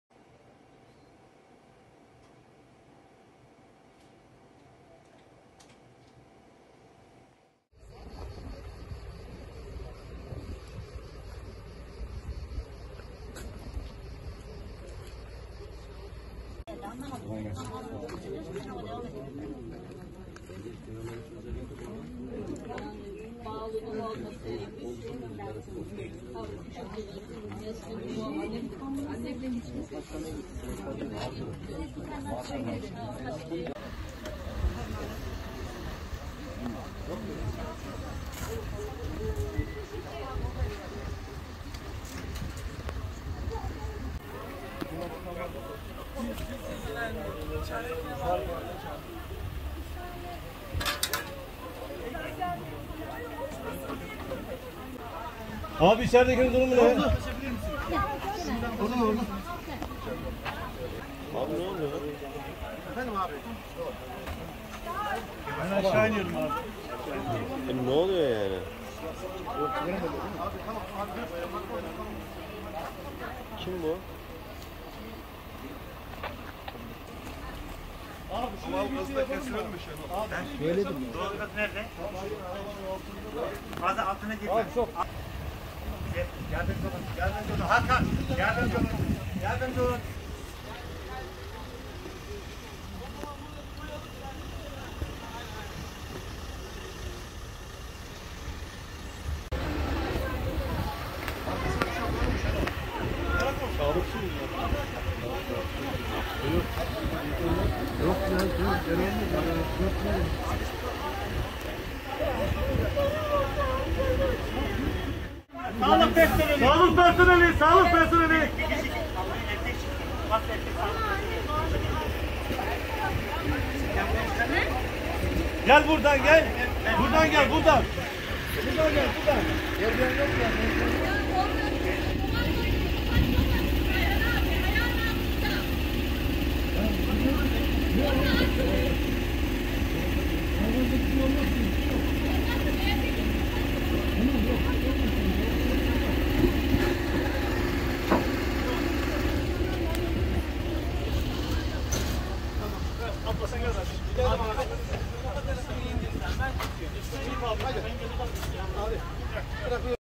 Sinop'ta trafik kazası 1 ölü 3 yaralı
Sinop’ta kontrolden çıkan otomobilin bir evin bahçesine uçtuğu kazada 1 kişi hayatını kaybetti, 3 kişi yaralandı.